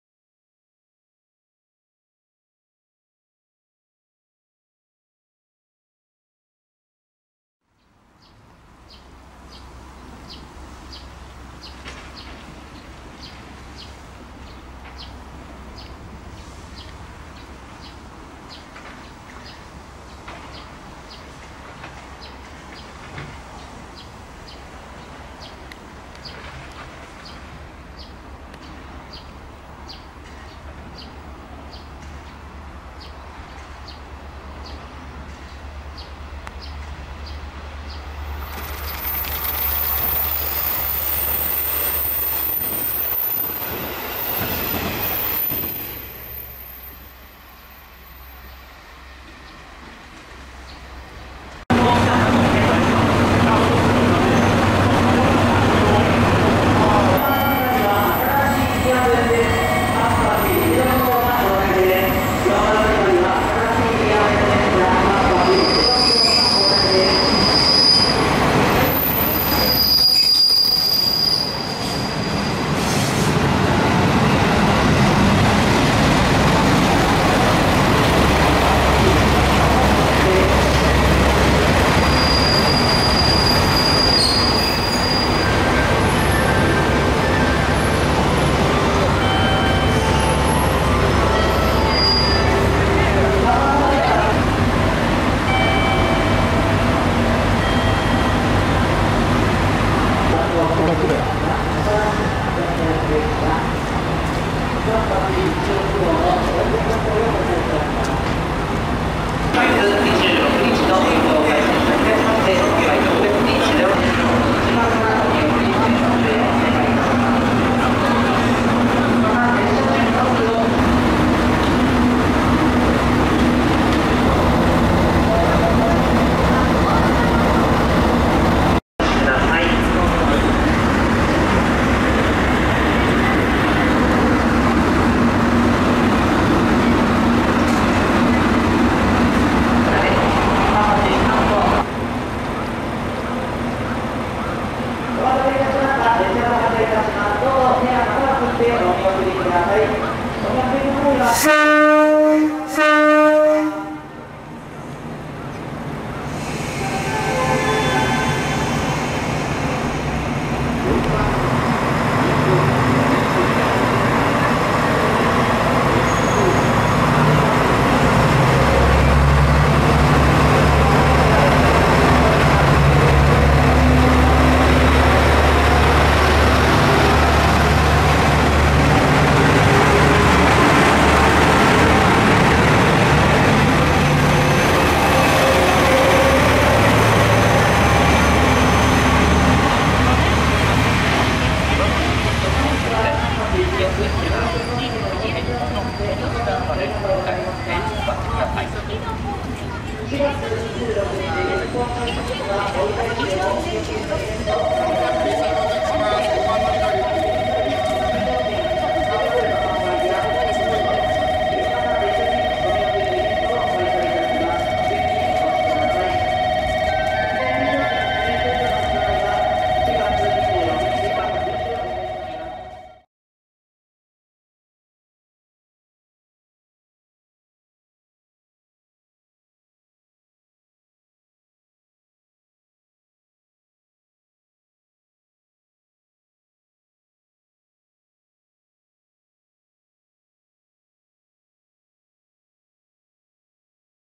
【新形式 2R形】かんぱち・いちろく 登場！ 沿線・大分駅
今月26日から、営業運転を開始する「かんぱち・いちろく」が小倉総合車両センターを出場し、大分車両センターに向いました。大分駅では車両見学会も開催され、多くの人で賑わいました。

改造元は、キハ47形で肥薩線の観光列車だった「いさぶろう・しんぺい」と普通列車として大分県内や唐津線等で使用されているキハ125形ですが、「2R
形」と言う全く新しい車両形式となっています。

出場直後の貴重な走行シーンと大分駅での見学会の様子をご覧下さい。